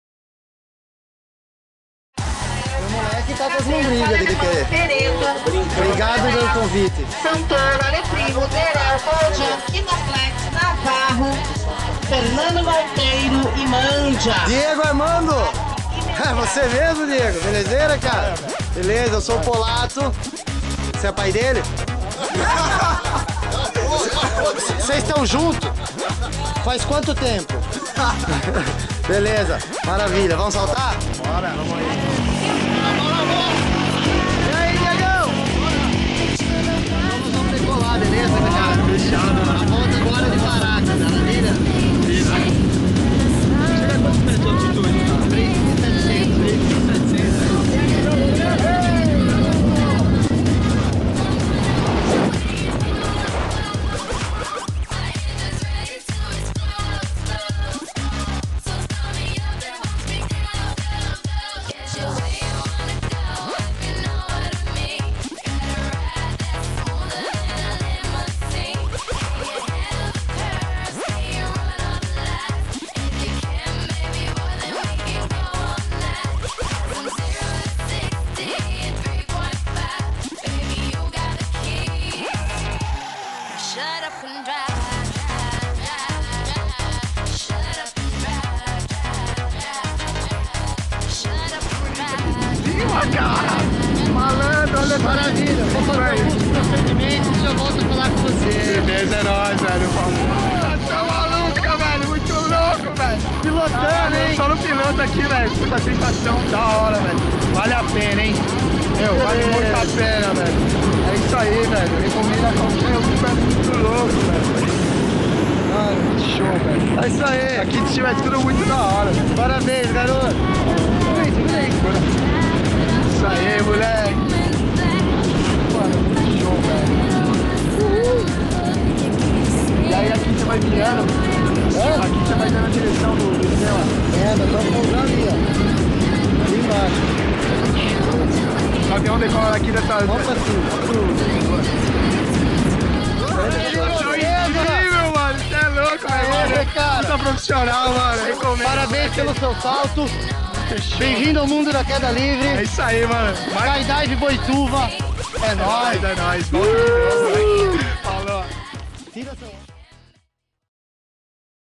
Salto duplo DIEGO ARMANDO 11/12/2010 (Handycam)
Salto duplo reaizado na escola Skydive Boituva com o instrutor Polato